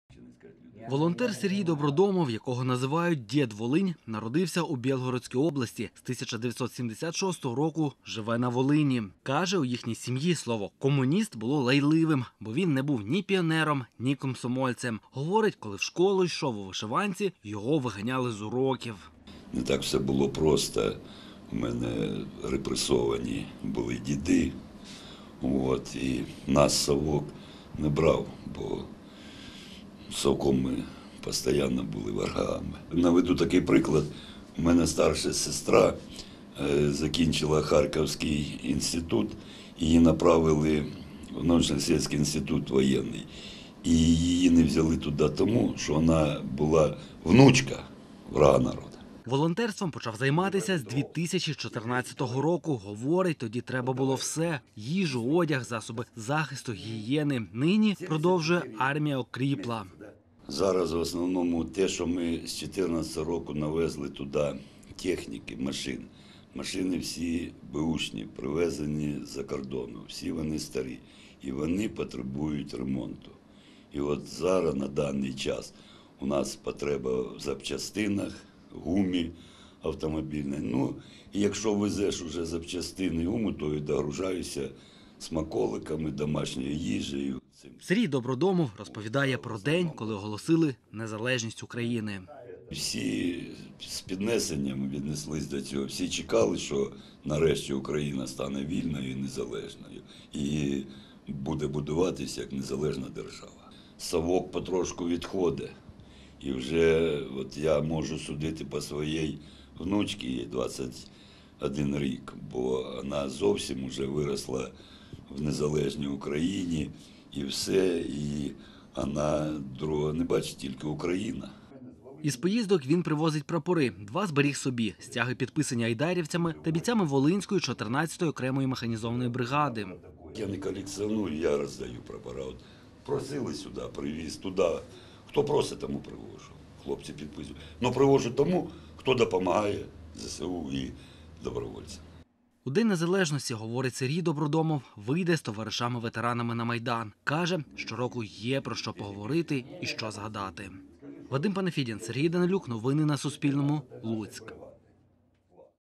Волинянин Сергій Добродомов, сьомий рік поспіль їздить на передові позиції українських військових
Волонтерством почав займатися з 2014-го року. Говорить, тоді треба було все: їжу, одяг, засоби захисту, гігієни. Нині, продовжує, армія окріпла.